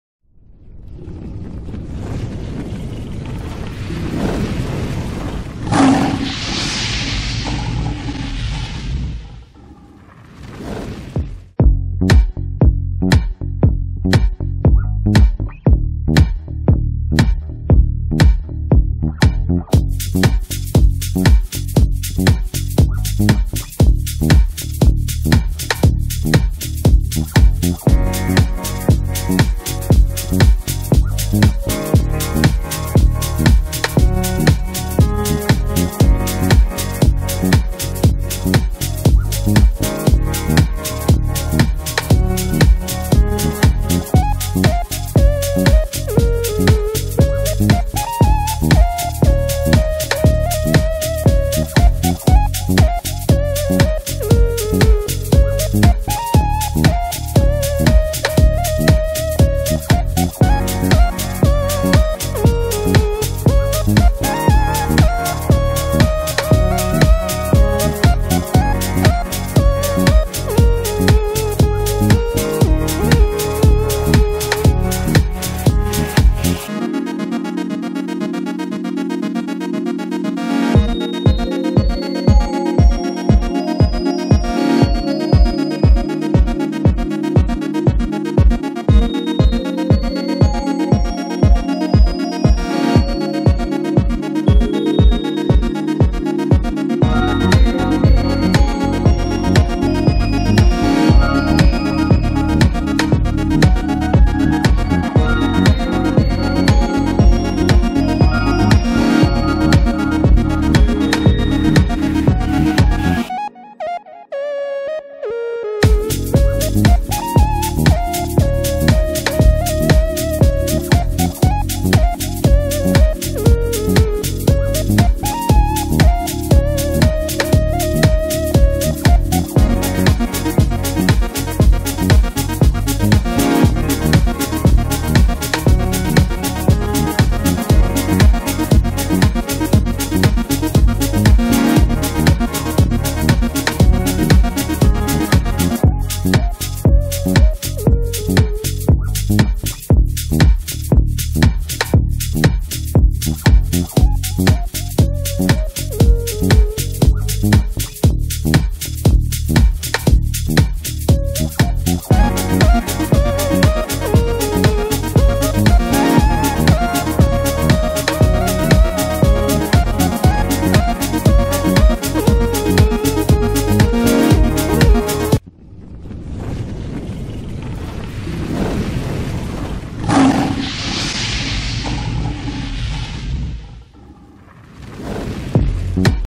2020 Subaru Outback vs 2019 Lexus NX (technical comparison)
Welcome to the video comparison between 2020 Subaru Outback vs 2019 Lexus NX. Please note that this is only a technical comparison, based solely on technical data of both cars. Not a real-life Subaru Outback vs Lexus NX review.